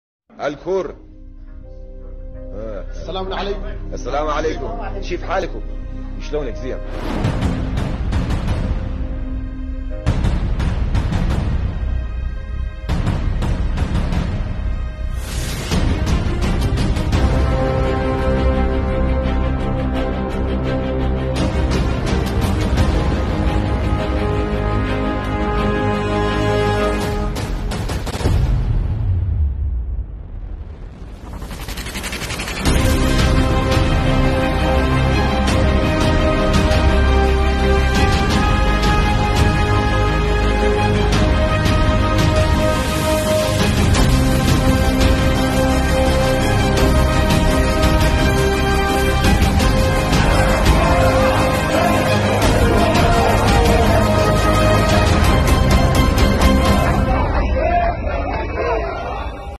انتفاضة الكرامة في الاحواز / اليوم السابع
انتفاضة الكرامة في الاحواز
انتفاضة الشعب العربي الاحوازي ضد سياسات ايران للتطهير العرقي في الاحواز
التيار الوطني العربي الديمقراطي في الاحواز